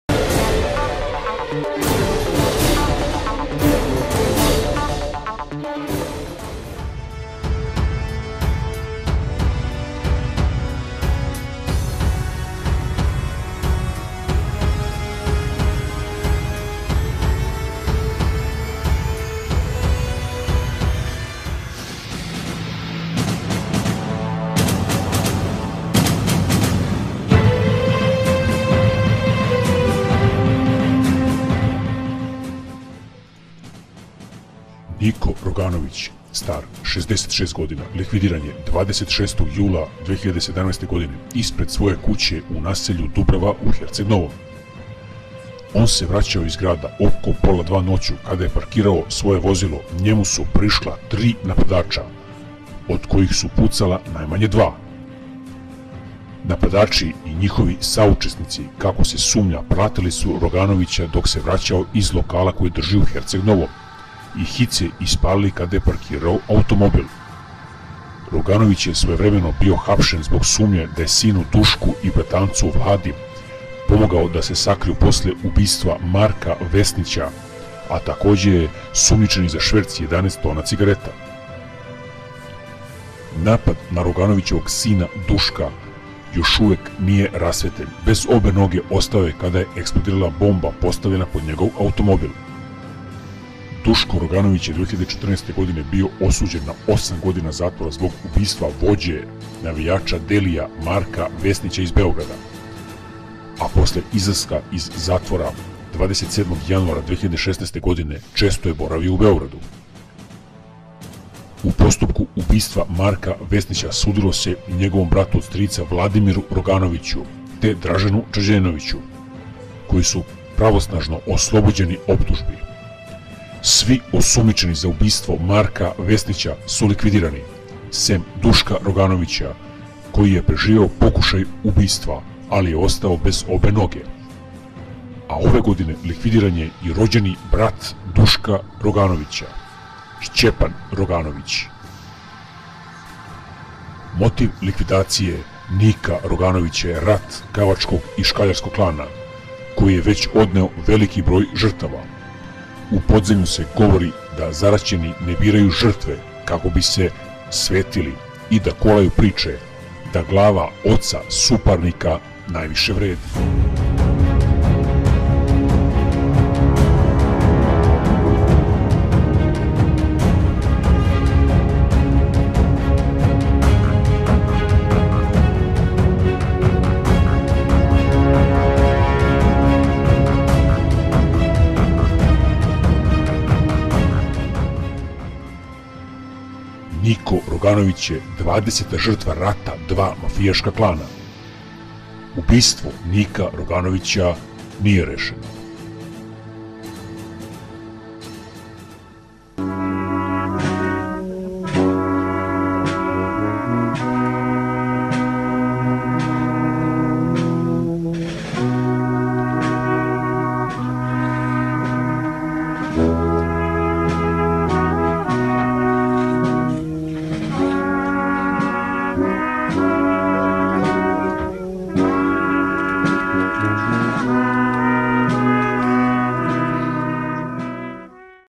NIKO ROGANOVIĆ(66) 26.07.2017
LIKVIDIRAN OTAC VOĐE KAVAČKOG KLANA: Niko Roganović likvidiran u sačekuši!
iko Roganović (66), otac jednog od vođa „kavačkog klana“ Duška Roganovića, koji je u podmetnutoj eksploziji 25. aprila ove godine ostao bez obe noge, nova je žrtva rata dva zavađena crnogorska klana, sumnjaju istražitelji iz susedne države.
Prema prvim informacijama iz istrage, Roganoviću su oko 1.20 posle ponoći ispred kućnog praga u naselju Dubrava u Herceg Novom prišla tri napadača, od kojih su pucala najmanje dva.
- Prišli su automobilu u kojem je bio i u njegovom pravcu ispalili više hitaca - naveli su izvori iz istrage. Napadači i njihovi saučesnici, kako se sumnja, pratili su ga dok se vraćao iz lokala koji drži u Herceg Novom i hice ispalili kada je parkirao automobil.
Prema nezvaničnim informacijama, ubijeni je nedavno primetio nepoznate osobe koje su pokušale da ga napadnu i o tome obavestio policiju, koja je uzela i snimke sigurnosnih kamera.
- Najverovatnije je motiv likvidacije rat „kavačkog“ i „škaljarskog klana“ koji je već odneo veliki broj žrtava. U podzemlju se govori da zaraćeni ne biraju žrtve kako bi se svetili i da kolaju priče da „glava oca suparnika najviše vredi“ - otkriva izvor upućen u istragu.
Ubistvo oca Duška Roganovića, kako se sumnja, nastavak je obračuna „kavačkog“ i „škaljarskog klana‘. Duško je 2014. bio osuđen na osam godina zatvora zbog ubistva vođe navijača Delija Marka Vesnića iz Beograda, a posle izlaska iz zatvora 27. januara 2016 godine često je boravio u Beogradu. Navodno je prijatelj s Belivukom, a nije tajna ni da je bio prijatelj pokojnog Saleta Mutavog - pisali su mediji.
Gotovo u isto vreme kada su nepoznati napadači pokušali da ga ubiju u Herceg Novom, u Beogradu je uhapšen njegov brat od strica Vladimir Roganović(KASNIJE JE I ON UBIJEN) zbog falsifikovanja dokumenata. Prema pisanju medija, istražitelji su došli do informacija da je „kavački klan“, u kom su braća Roganović, pripremao likvidaciju u Beogradu i da su upravo Roganovići imali ulogu u tome. Kako se sumnjalo, „škaljarci“ su to saznali i preduhitrili ih tako što su pokušali da ubiju Duška.
Dva najjača kriminalna klana s kojima pokušavaju da se izbore srpska i crnogorska policija, „kavački“ i „škaljarski klan“, poreklom su iz Crne Gore. To je u početku bila jedna kriminalna grupa, sve do 2013, kada je u Valensiji u Španiji nestalo 300 kilograma kokaina, nakon čega se grupa podelila. Rat dva klana se iz Kotora proširio i na Beograd i Novi Sad, a navodno za likvidacije oba koriste napadače iz Bosne i Hercegovine.
UBICA N.N